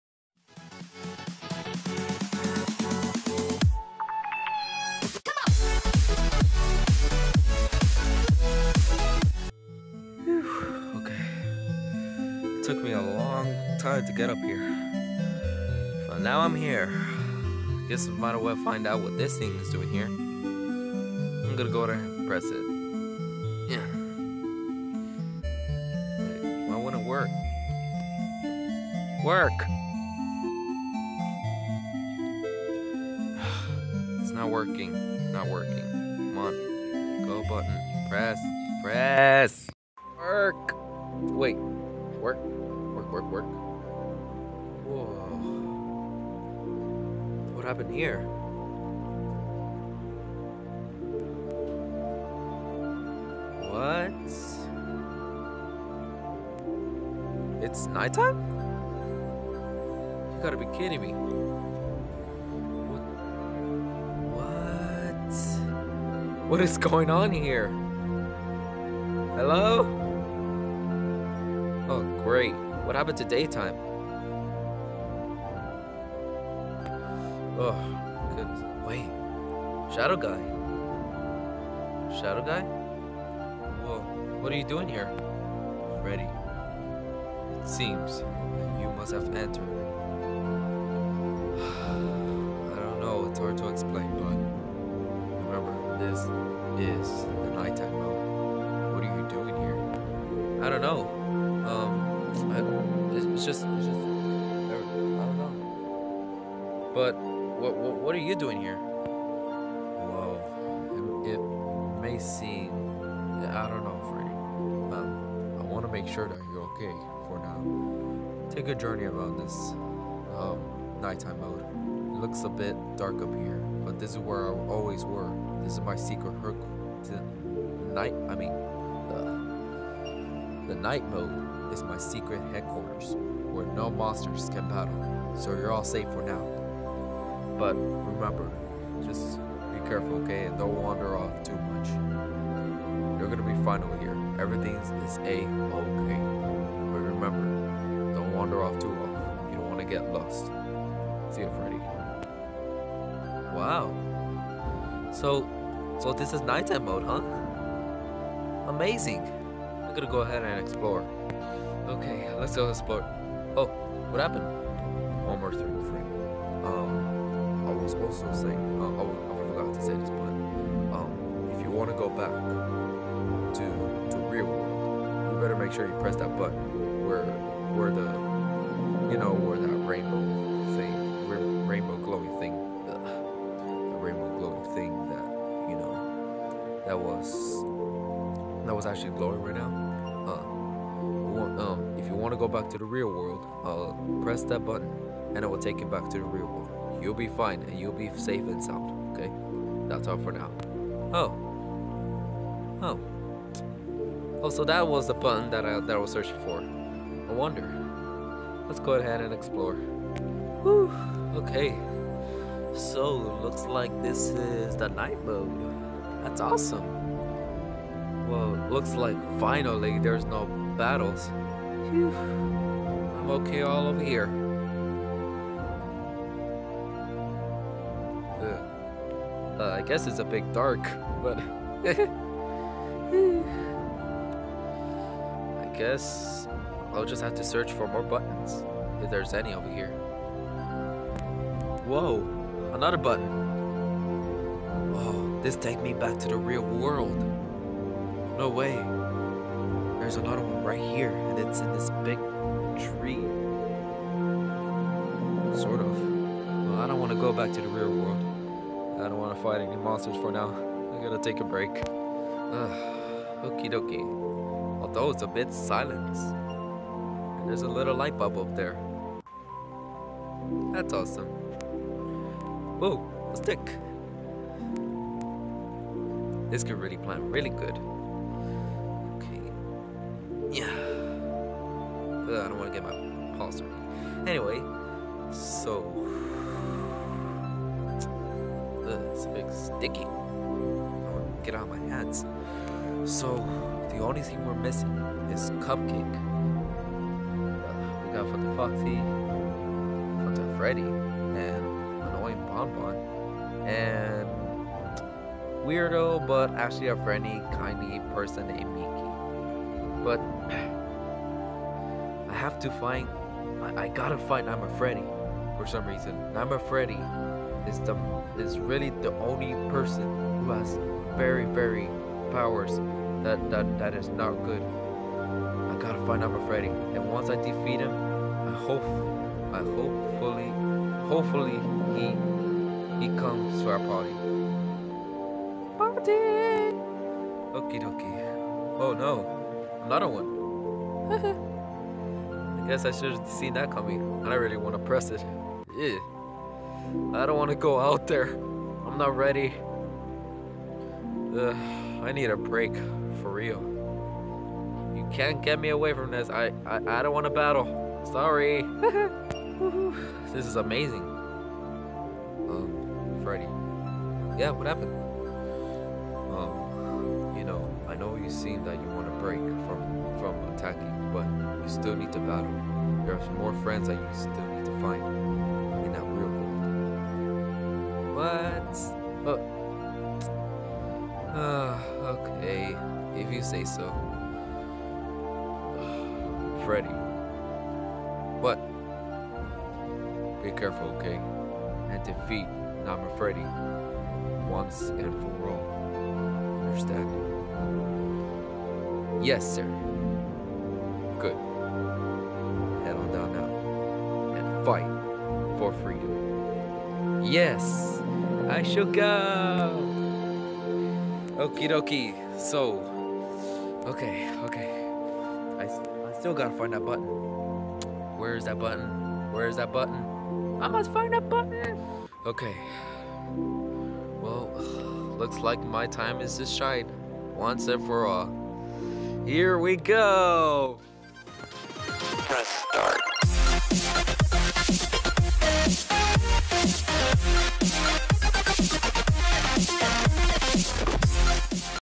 Fnaf World Series Season 1 - Episode 5
Woah there, looks like Freddy found a way into the night world. But will Freddy find all of his friends as soon as possible before N. Freddy attacks? Find out nextime